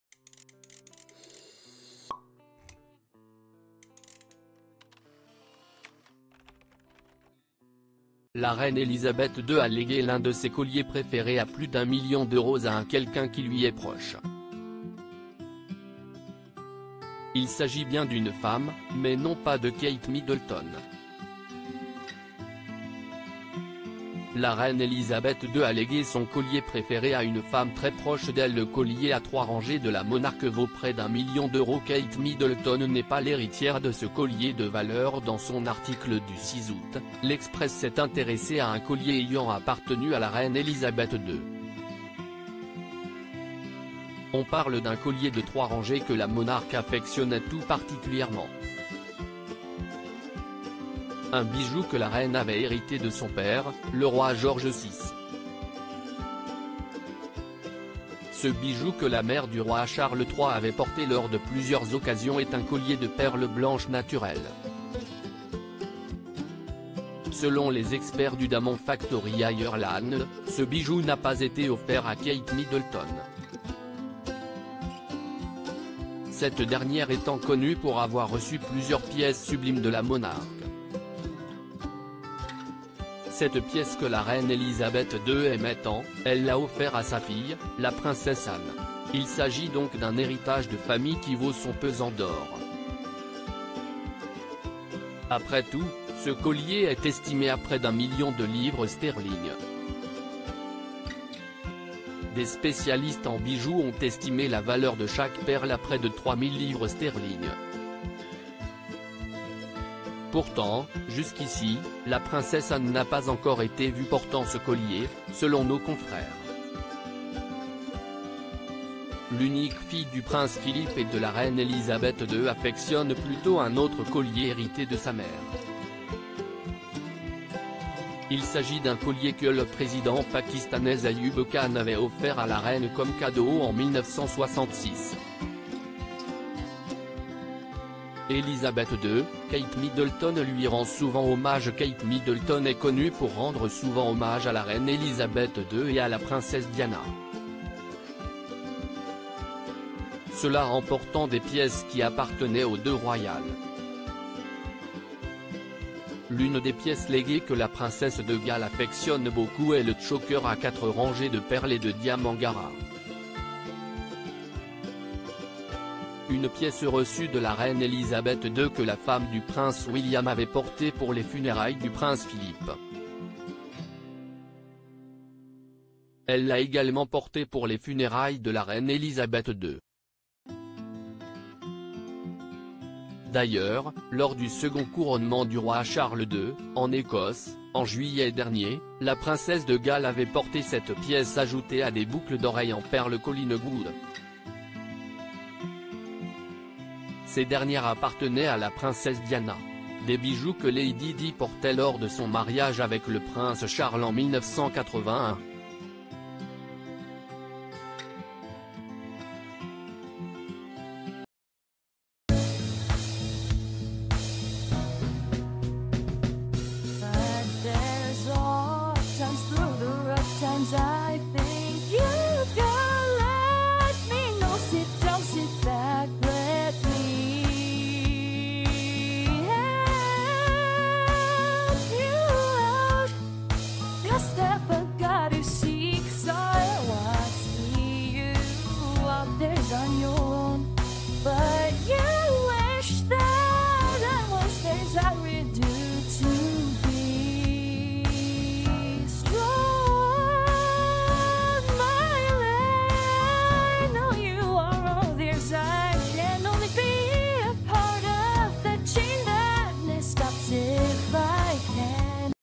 Elizabeth II : ce collier de perles à plus d’un million d’euros légué à une femme de la famille roya
Elizabeth II : ce collier de perles à plus d’un million d’euros légué à une femme de la famille royale